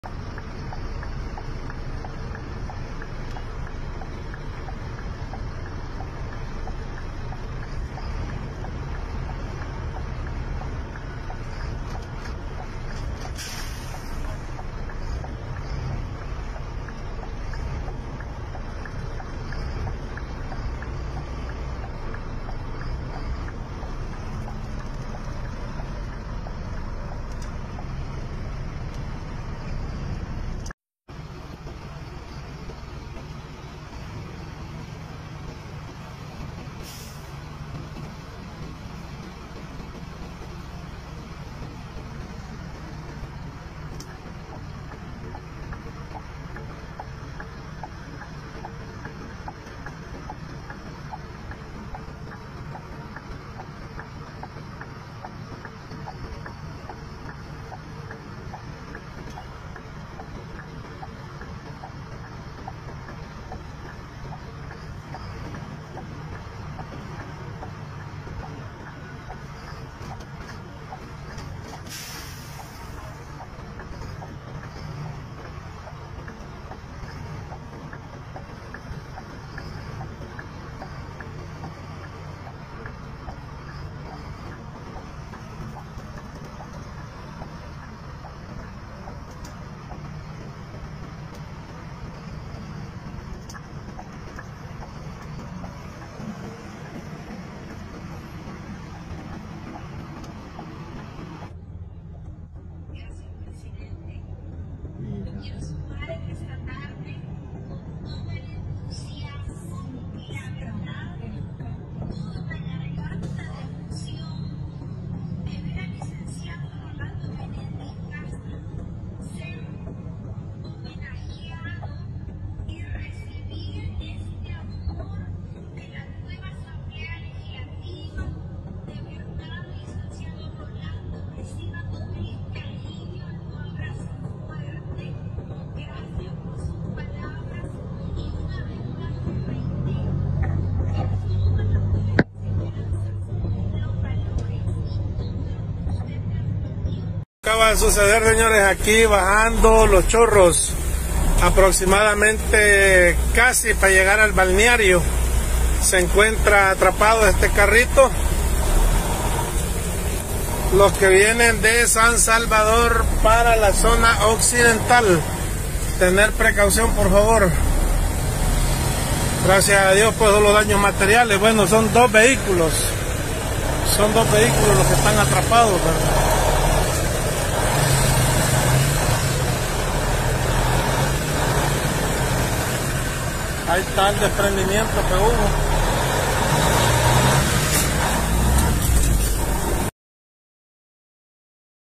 Derrumbe en Los Chorros
Reportan derrumbe este martes en la Zona de Los Chorros.